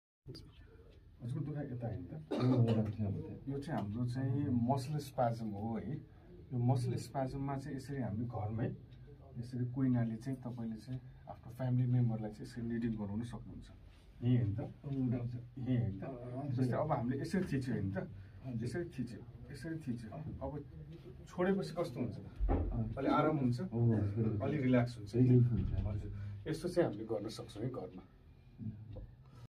upper back pain can be treated by deep tissue at home also
deep tissue can help a lot for upper back pain symptoms due to spasms